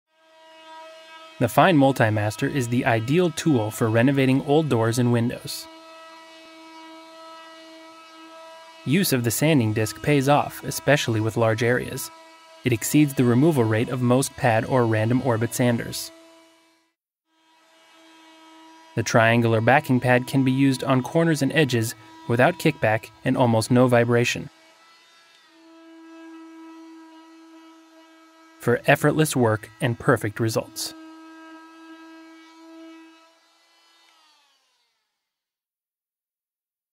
FEIN MultiMaster and corner sanding
Tackle tough corner sanding jobs with the FEIn MutliMaster and sander attachement.